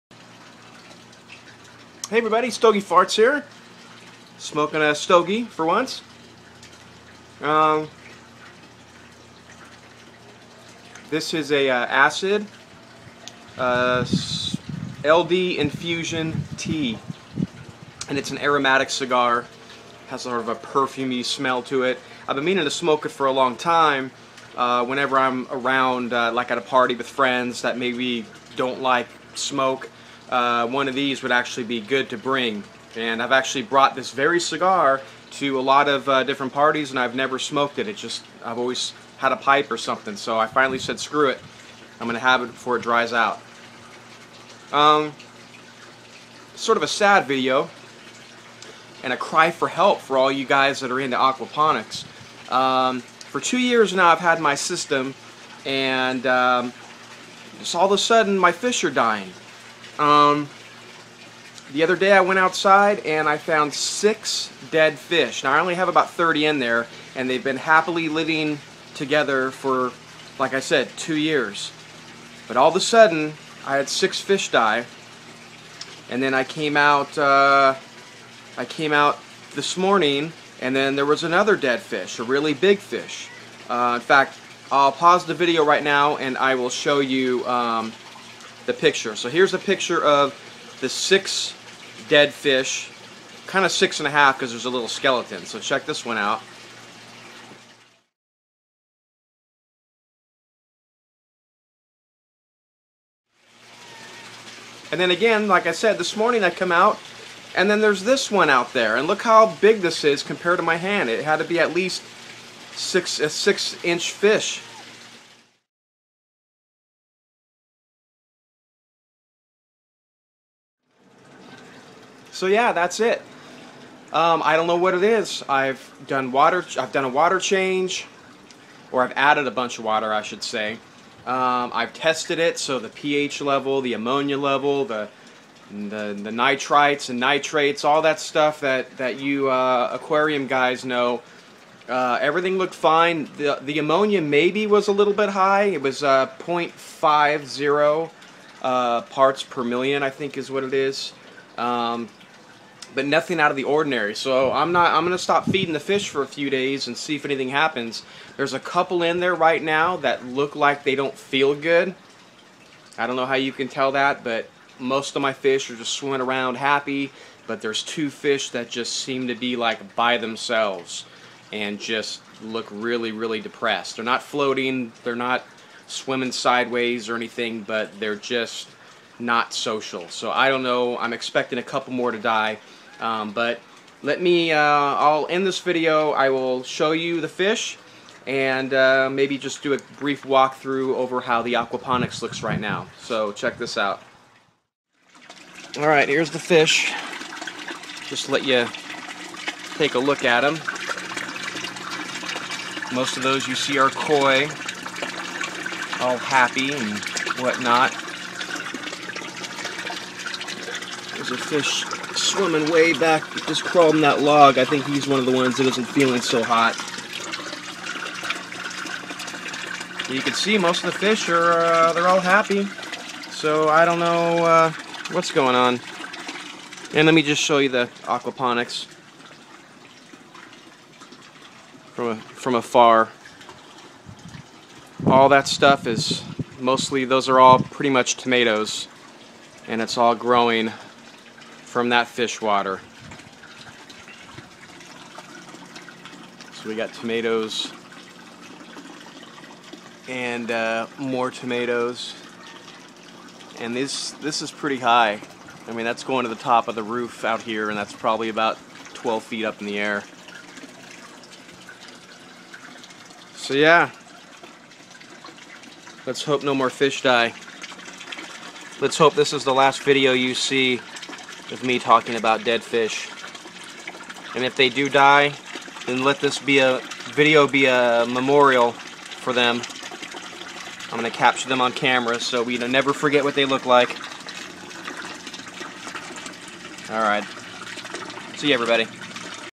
Cigar Smoking - HELP! My Aquaponics Fish are DYING!
While smoking a cigar, I wonder why all of my aquaponic fish are dying! They've been happy for two years now. all pH, ammonia, and nitrate levels seem good. The only thing I can think of is that perhaps the ammonia may have been slightly high (at 0.50 ppm), and aside from that, I recently tried to clean a lot of old fish poo which was sitting around the pump. so perhaps there was an undetectable ammonia spike or something (?). It's really frustrating to not know what suddenly changed, because I haven't done anything different to cause a mass die off like this.